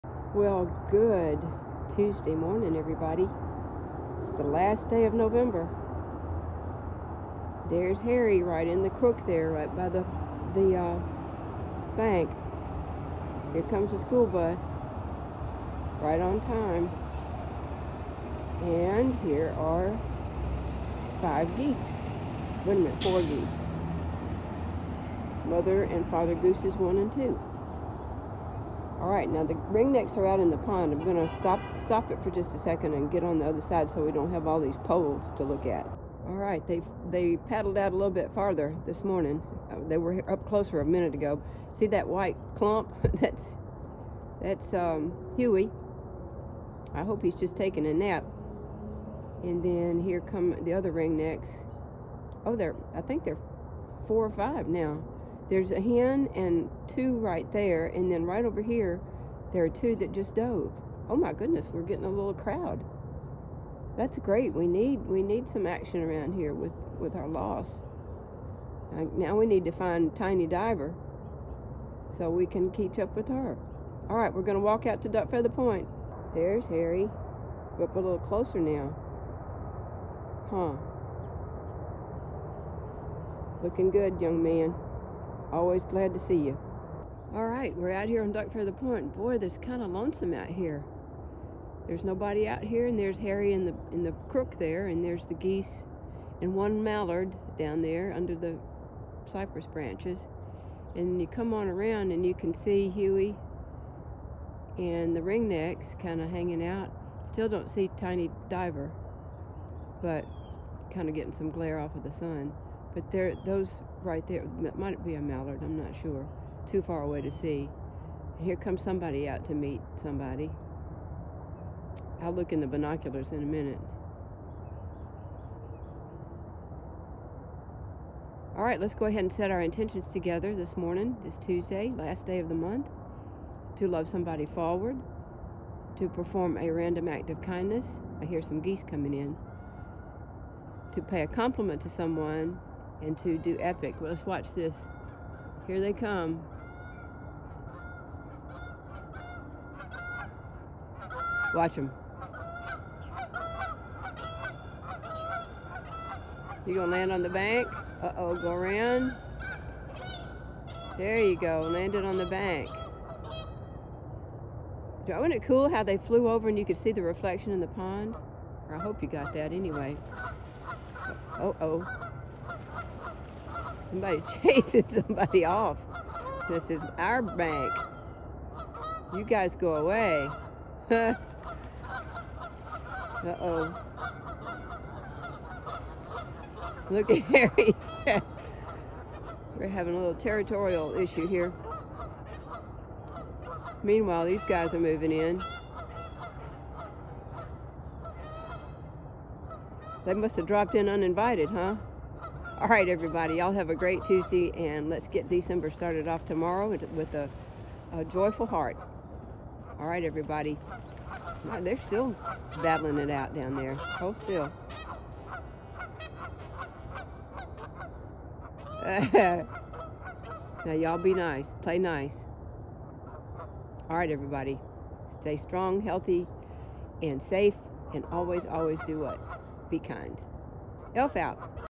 “Pondlandia is Thankful All Year ‘Round!”©
TODAY we say goodbye to November and the month in which we start holiday celebrations galore. We here in Pondlandia celebrate every day as a gift and today is no exception. With fall colors this year being the most vivid we’ve seen in years, we celebrate that we have been witness to that. We celebrate the life of Dewey whose companionship with Huey has been unrelenting and unwavering…until he could no longer. We celebrate Harry the Great Blue Heron for his faithfulness and ability to fish with the best of them. And then, there are the Mallards that have hung with us most of the year and have enjoyed the corn and the camaraderie of other feathered folk. At the beginning of the month, we celebrated the arrival of the Ring-necks and of Tiny diver who visit us regularly in the late fall. Thought we’ve not seen Casper the Great White Egret of late, we know he’s around; mayhap he eschews too ‘chefs in the kitchen’. And we absolutely cannot but cherish little Barnes and Noble, the goslings that were with us for such a short time.

We simply adore this Pond and its inhabitants as it’s a microcosm of the world…all kinds of ‘folks’ needing all kinds of ‘space’; there are spats and fusses but in the end, things seem to settle down and all is right with the world yet again. We do love our Peaceable Kingdom and will strive to make this next month a fitting in to a year well lived. Elf Out!